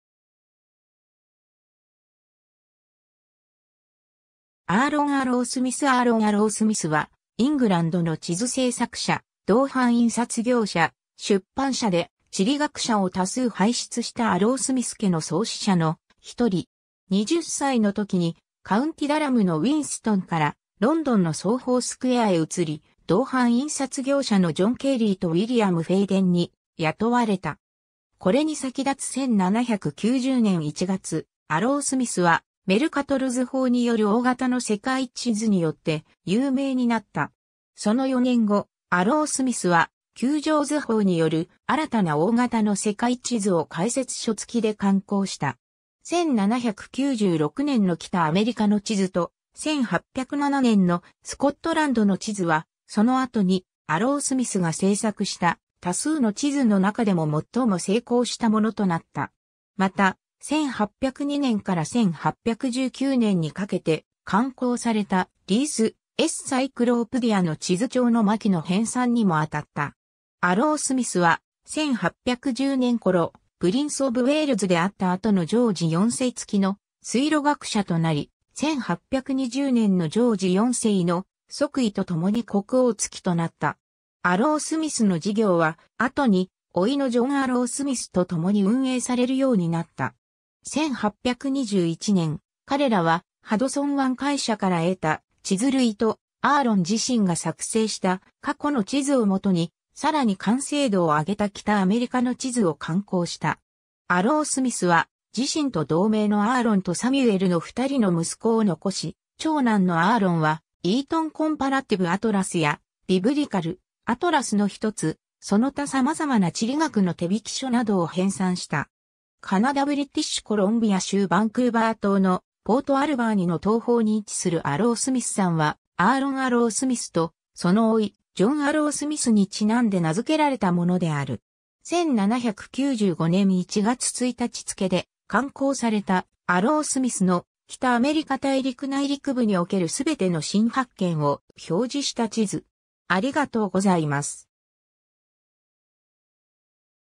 アーロン・アロースミス
アーロン・アロースミス アーロン・アロースミス（英: Aaron Arrowsmith, 1750年 – 1823年）は、イングランドの地図製作者、銅版印刷業者、出版者で、地理学者を多数輩出したアロースミス家の創始者のひとり。
 20歳のときに、カウンティ・ダラムのウィンストン (Winston) からロンドンのソーホー・スクエア (Soho Square) へ移り、銅版印刷業者のジョン・ケーリー (John Cary) とウィリアム・フェイデン (William Faden) に雇われた。
 これに先立つ1790年1月、アロースミスはメルカトル図法による大型の世界地図によって有名になった。
その4年後、アロースミスは、球状図法 (globular projection) による新たな大型の世界地図を解説書付きで刊行した。
1796年の北アメリカの地図と、1807年のスコットランドの地図は、その後にアロースミスが製作した多数の地図の中でも最も成功したものとなった。
また、1802年から1819年にかけて刊行された『Rees's Cyclopædia』の地図帳の巻の編纂にもあたった。
 アロースミスは、1810年ころ、プリンス・オブ・ウェールズであった後のジョージ4世付きの水路学者となり、1820年のジョージ4世の即位とともに国王付きとなった。
 アロースミスの事業は、後に甥のジョン・アロースミス（1790年 - 1873年）とともに運営されるようになった。
1821年、彼らはハドソン湾会社から得た地図類と、アーロン自身が作成した過去の地図をもとに、さらに完成度を上げた北アメリカの地図を刊行した。
 アロースミスは、自身と同名のアーロンとサミュエルの2人の息子を残し、長男のアーロンは『Eton Comparative Atlas』や、『Biblical atlas』のひとつ、その他様々な地理学の手引書などを編纂した。
 カナダ・ブリティッシュコロンビア州バンクーバー島のポート・アルバーニ (Port Alberni) の東方に位置するアロースミス山 (Mount Arrowsmith) は、アーロン・アロースミスと、その甥ジョン・アロースミスにちなんで名付けられたものである。
 1795年1月1日付で刊行された、アロースミスの「北アメリカ大陸内陸部におけるすべての新発見を表示した地図 (Map Exhibiting All the New Discoveries in the Interior Parts of North America)」